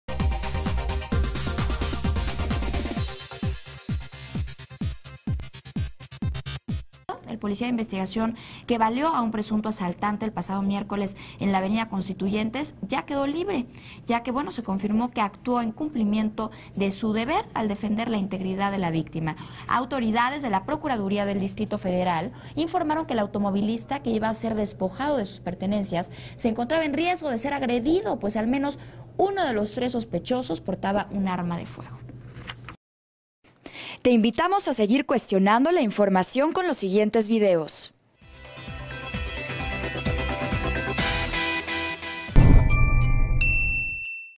Liberan a policía que baleó a asaltante en Constituyentes / Comunidad
El agente de investigación que frustró el robo de vehículo y está relacionado con tres averiguaciones previas por robo de relojes en calles de la delegación Miguel Hidalgo.

12 de junio 2015